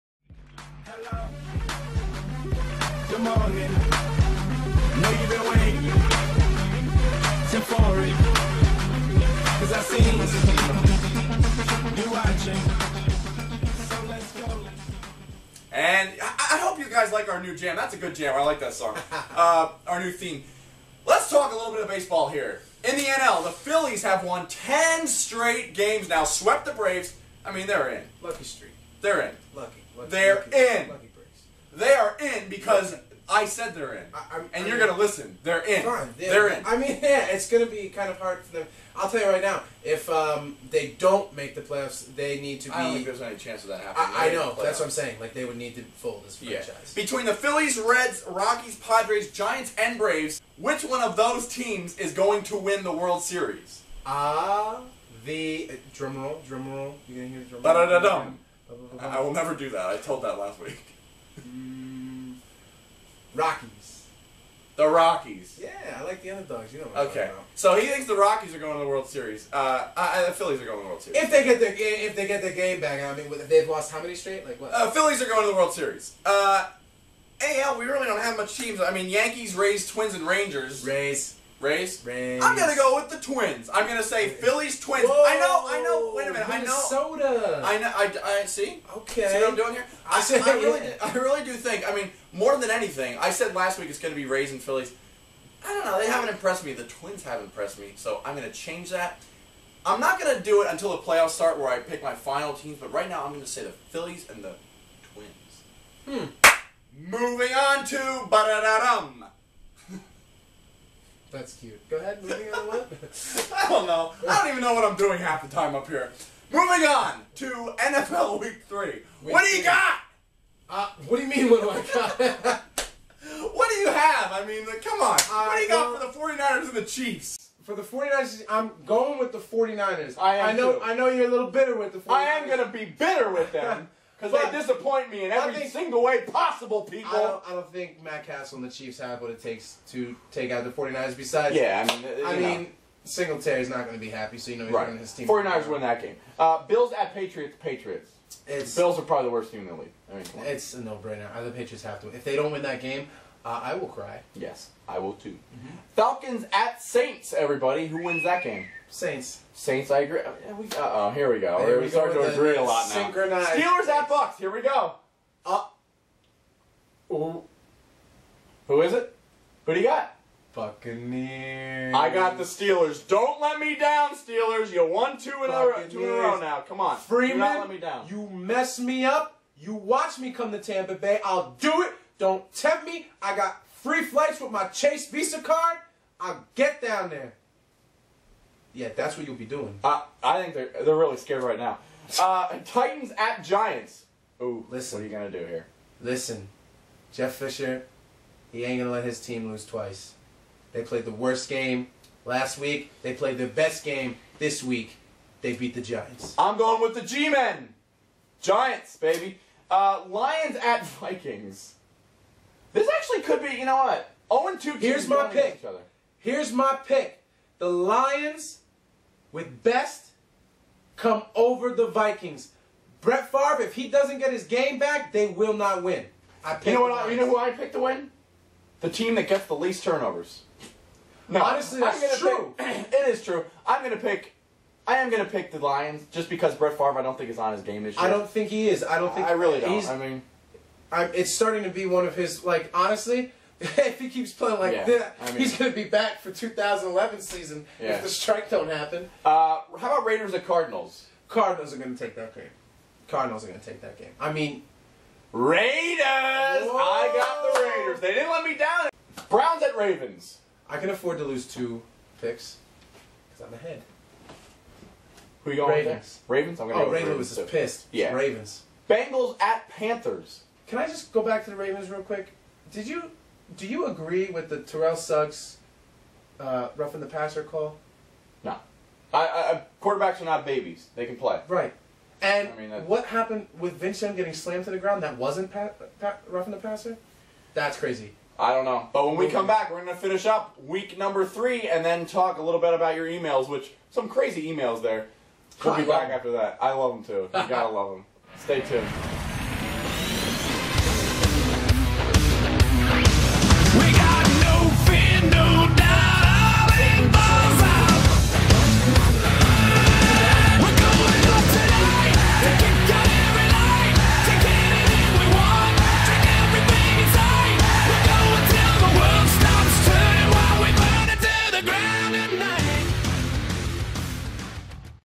Sports Talk - Episode 3 - Part 2 - NFL Week 3 and World Series Predictions!
Episode three, part 1 of the "Sports Talk" show. Week 2 review, Week 3 Preview/Predictions, and World Series predictions!

We will ask you questions on our Live show! and Include you in our Youtube talk segment!